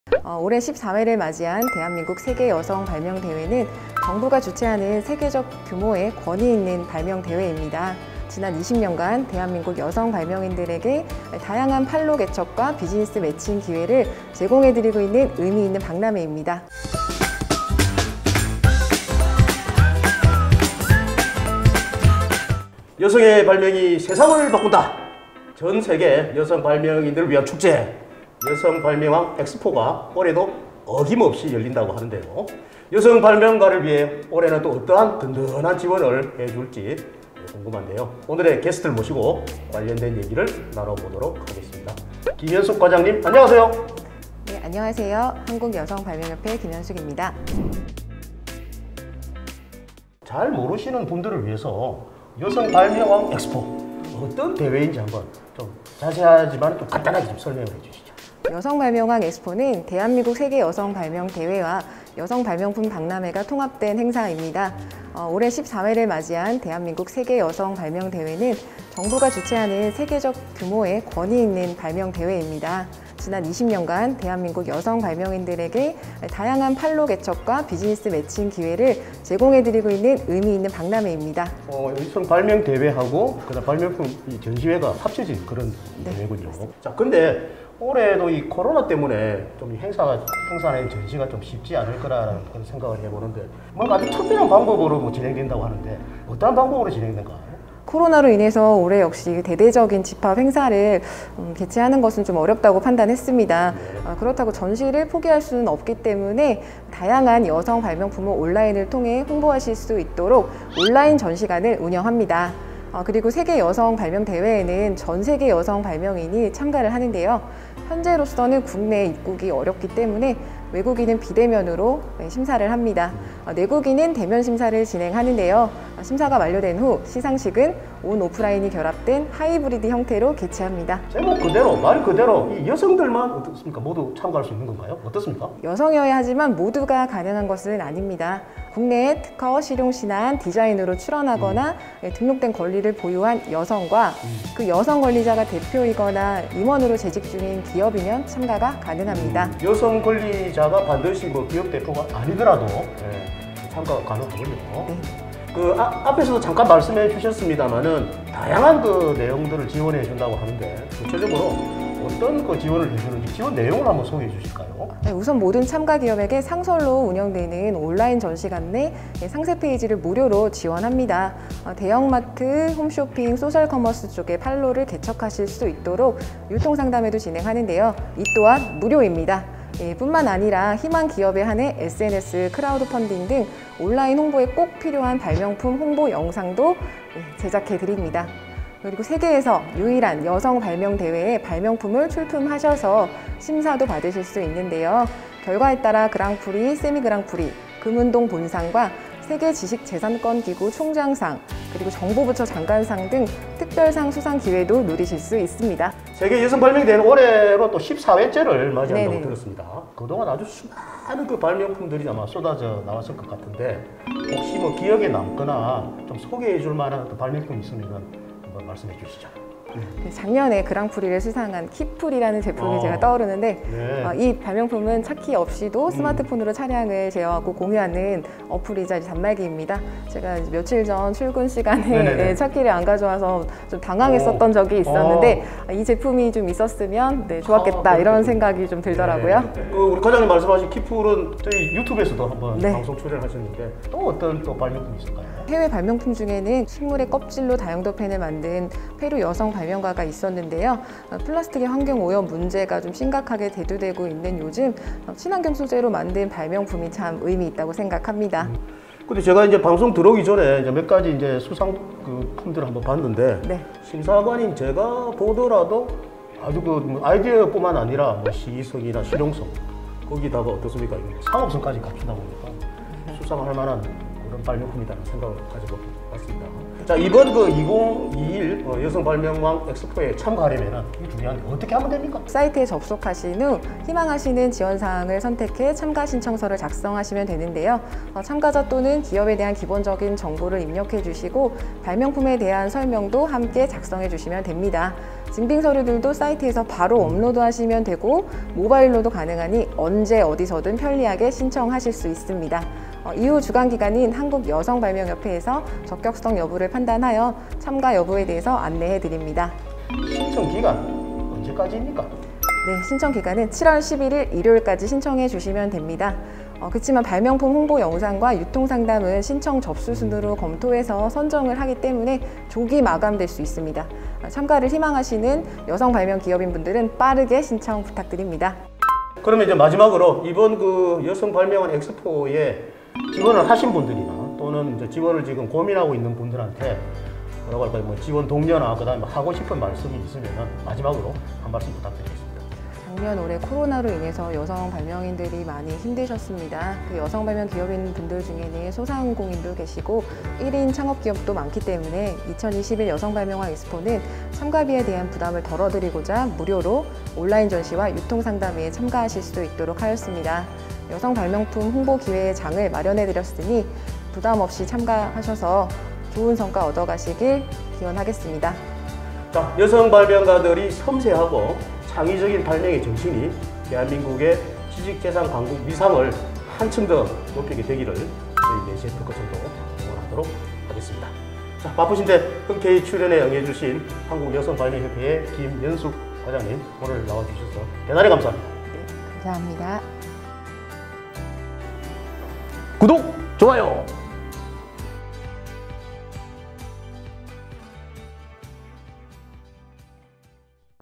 여성발명왕 EXPO에 참가하세요! 참가신청으로 홍보, 판로 개척, 영상 제작까지 해주는 대회가 있다?(Feat.2021 여성발명왕EXPO)
세계에서 유일한 여성발명대회, 여성발명왕EXPO!

여성발명왕EXPO 참가 조건부터 혜택이 궁금하신가요?

한국여성발명협회 김연숙 과장님께 직접 들었습니다.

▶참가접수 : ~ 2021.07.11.

「4시! 특허청」  408회
게스트 :   한국여성발명협회 김연숙 과장